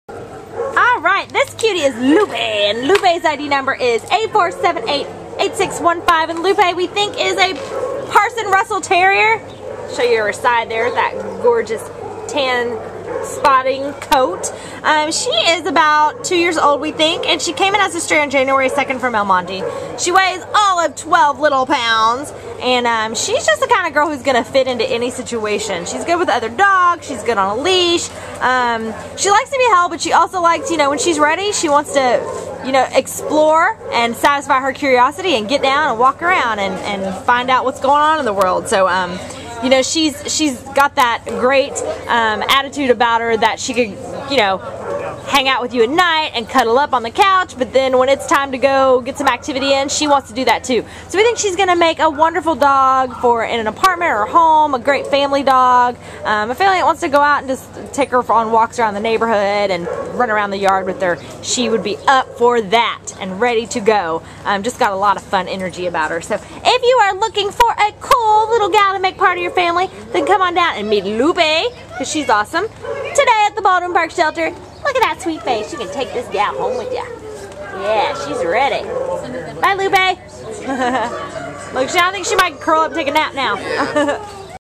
A4788615 Lupe
A4788615 Lupe is a gregarious, confident 2-year-old white-and-tan female Parson Russell Terrier mix who was found in El Monte on January 2nd and brought to the Baldwin Park Animal Care Center. Weighing 12 lbs, the lovely Lupe enjoys being held but—get ready—prefers to walk, even in the shelter environment! And she walks really well on leash, seemingly ready to just walk out the door with a new family and get to some good family living. Lupe also does well with other dogs—both her own size and the big ones; she takes whatever and whoever comes along in stride, curious and confident at the same time. She has moderate energy, so she’ll likely excel at activities (including playing) and lounging with you while you read, surf the Internet, or watch TV. We think she’ll be an excellent indoor pet for any individual or family who wants an assured little lady.